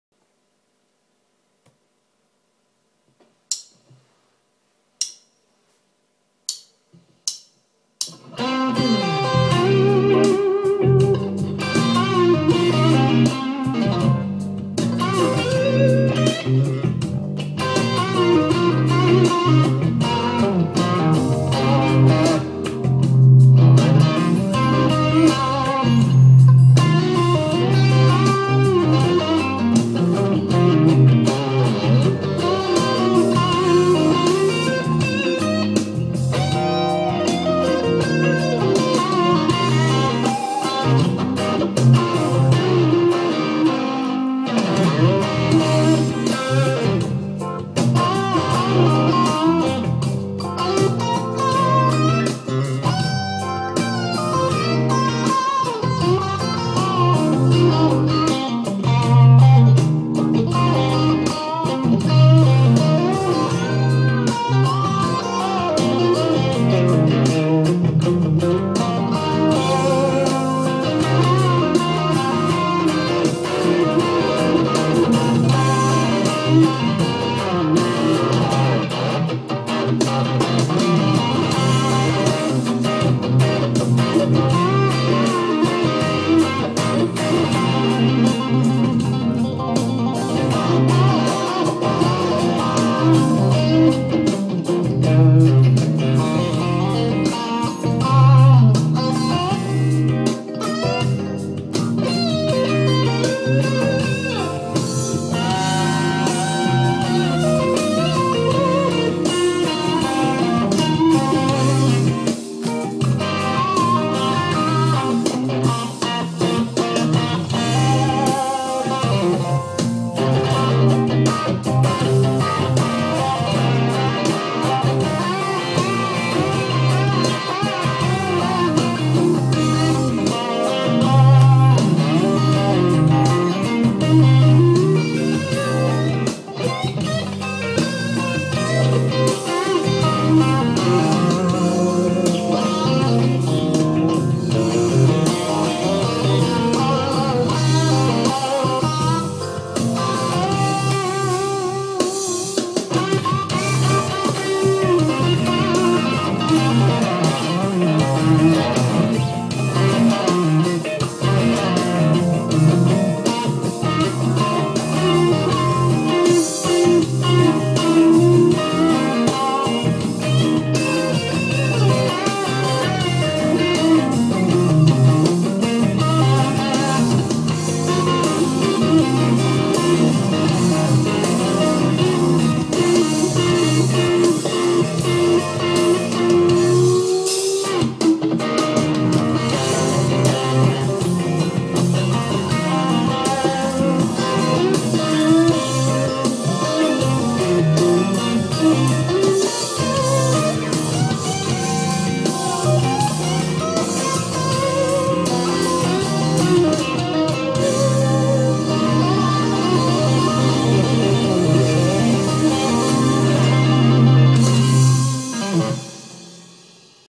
Blues guitarist Michael Fredericks (Filthy Gorgeous)
Trying out my new Texas specials